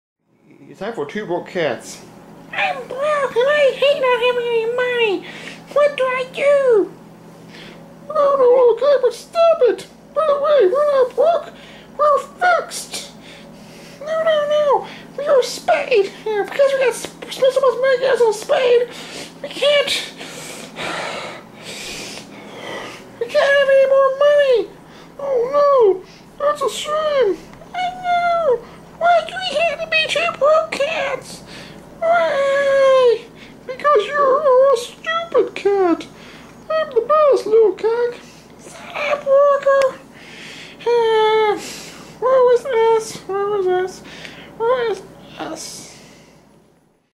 Two Broke Cats
It's a show about two broke cats who hate being broke!